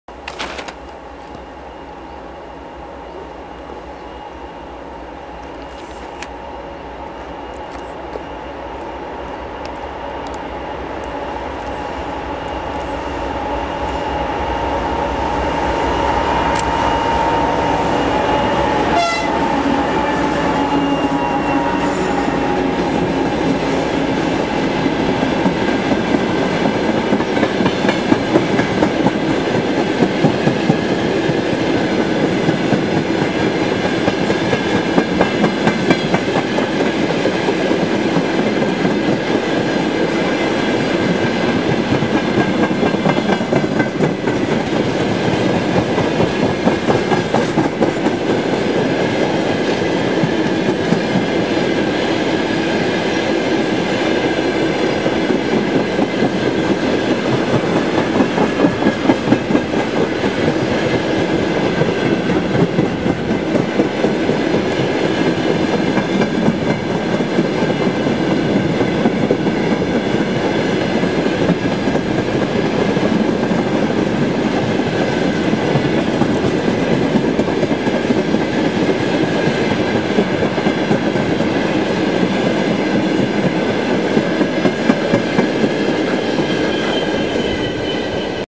Тепловоз 2ТЭ116-1689 близ о.п. Янеда / 2TE116-1689 between Nelijärve and Jäneda
Russian 2TE116-1689 between Nelijärve and Jäneda, Estonia, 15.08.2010